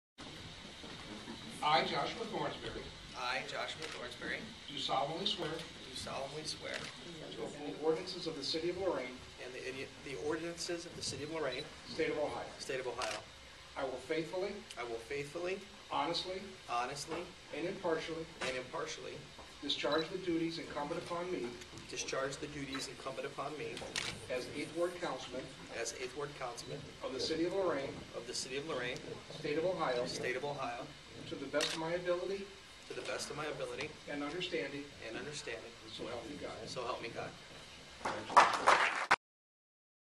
Lorain Councilman-elect Joshua Thornsberry takes his oath of office with his son, Hunter, 5, holdin
Lorain Councilman-elect Joshua Thornsberry takes his oath of office with his son, Hunter, 5, holding the Bible.